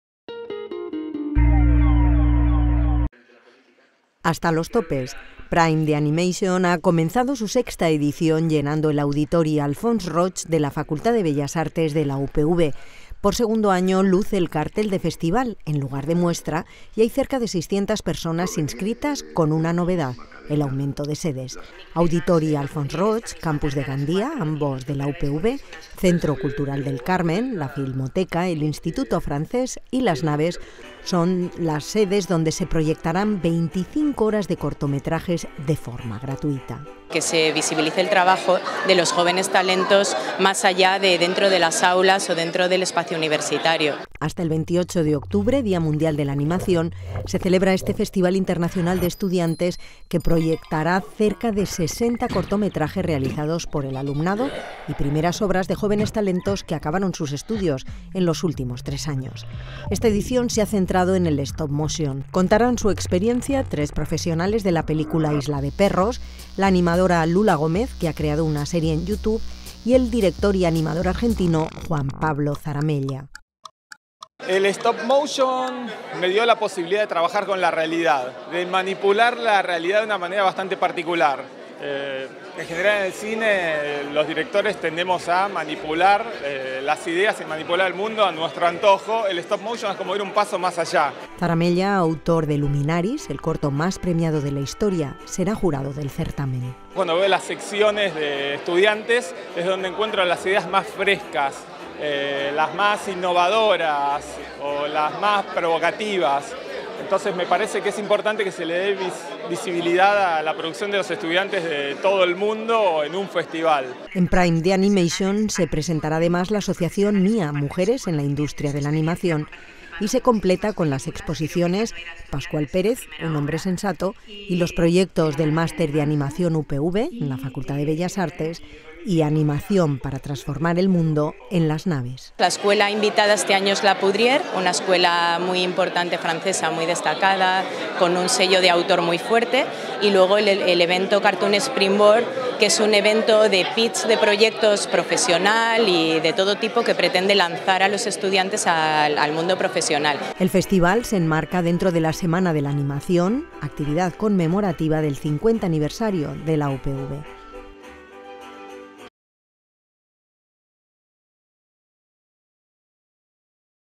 Prime the Animation 6! en la FBBAA-UPV - Noticia @UPVTV, 26-10-2018
Hasta los topes. Prime the Animation ha comenzado su sexta edición llenando el Auditori Alfons Roig de la Facultad de Bellas Artes de la UPV. Por segundo año luce el cartel de festival, en lugar de muestra, y hay cerca de 600 personas inscritas, con una novedad: el aumento de sedes. Auditori Alfos Roig, Campus de Gandia, ambos de la UPV, Centro Cultural del Carmen, la Filmoteca, el Instituto Francés y Las Naves son las donde se proyectarán 25 horas de cortometrajes, de forma gratuita.

Hasta el 28 de octubre, Día Mundial de la Animación, se celebra este Festival Internacional de Estudiantes que proyectará cerca de 60 cortometrajes realizados por el alumnado y primeras obras de jóvenes talentos que acabaron sus estudios en los últimos tres años. Esta edición se ha centrado en el stop motion. Contarán su experiencia tres profesionales de la película “Isla de perros”, la animadora Lula Gómez, que ha creado un aserie en you tube y el director y animador argentino Juan Pablo Zaramella.

Zaramella, autor de Luminaris, el corto más premiado de la historia, será jurado del certamen.

En Prime the Animation se presentará la Asociación MIA Mujeres en la Industria de la Animación y se completa, además, con las exposiciones "Pascual Pérez. Un hombre sensato", y los “Proyectos del Máster de Animación UPV”, en la Facultad de Bellas Artes y "Animación para transformar el mundo", en Las Naves.

El festival se enmarca dentro de la Semana de la Animación, actividad conmemorativa del 50 Aniversario de la UPV.